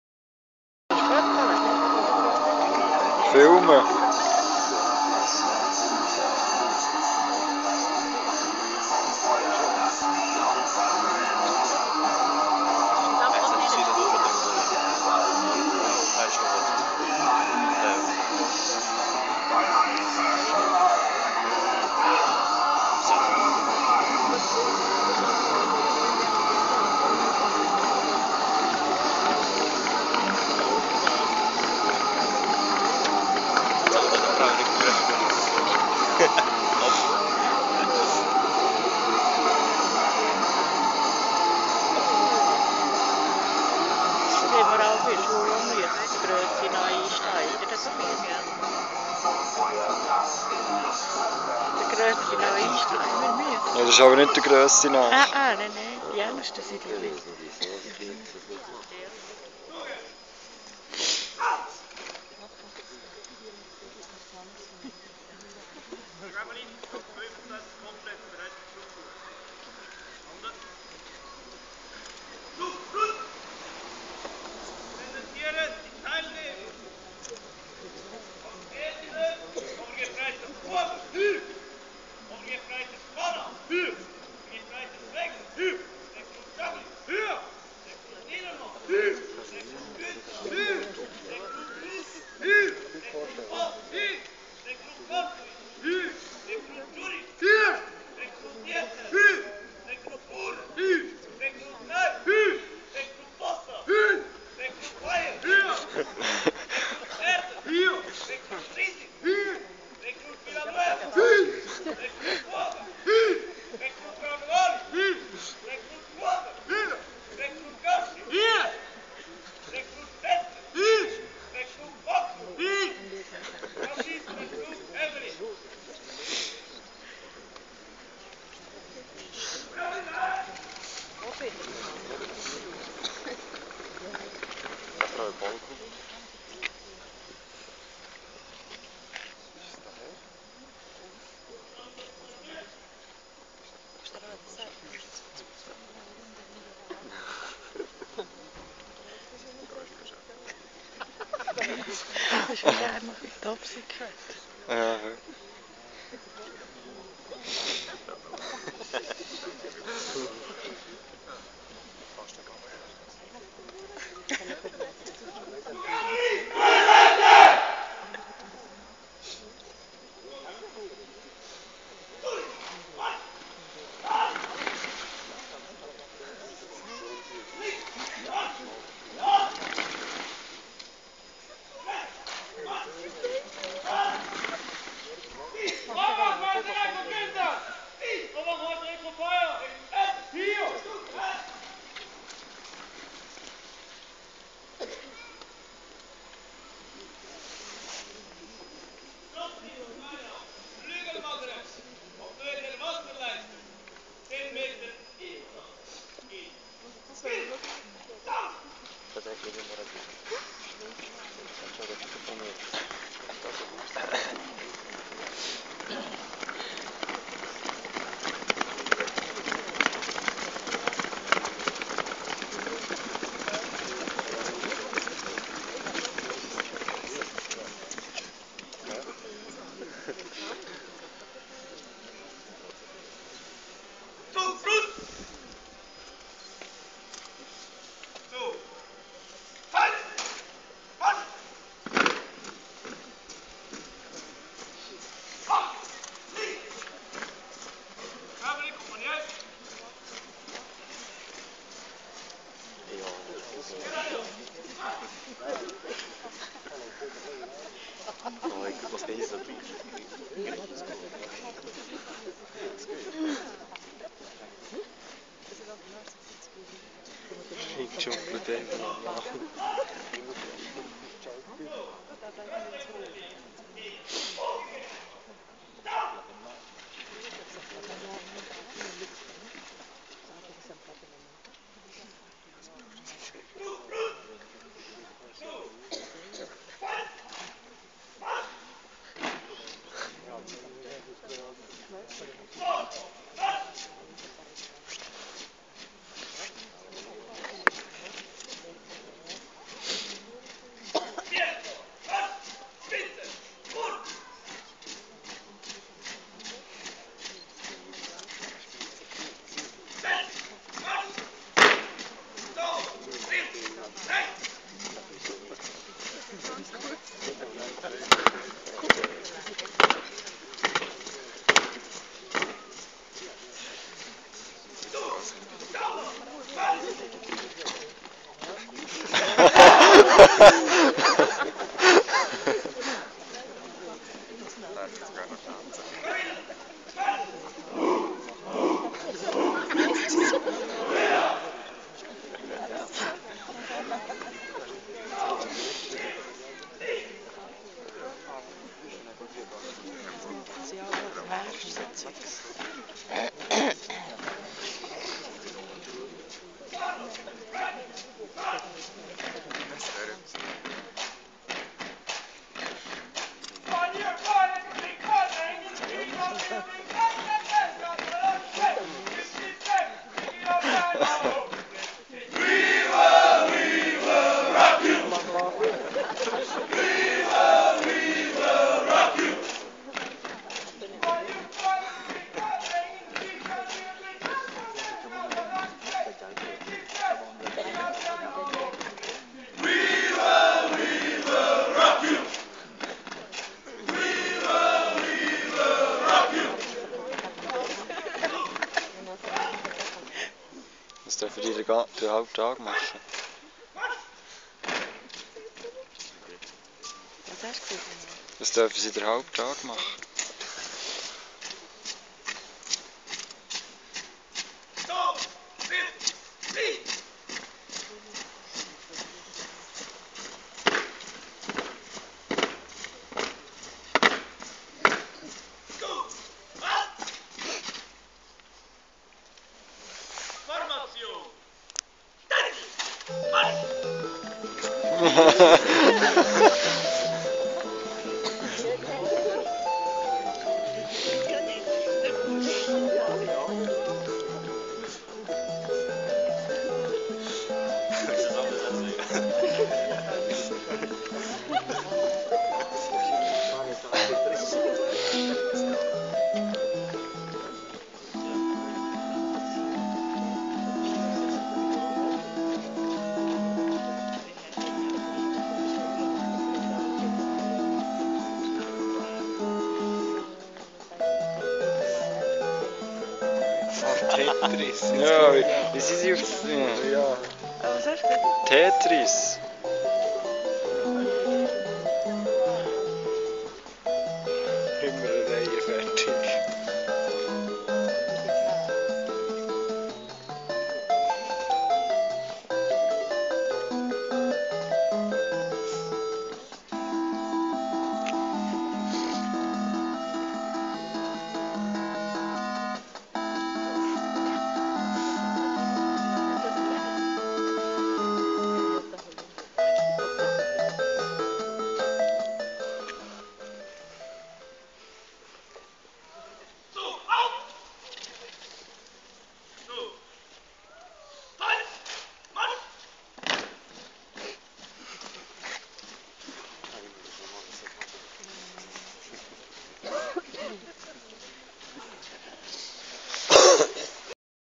Zugschule Zug Häfeli
Zugschule in Frauenfeld am Tag der Angehörigen am 18. Januar 2014. Vorgeführt vom Zug 3 der Übermittlungs- und Führungsunterstützungstruppen.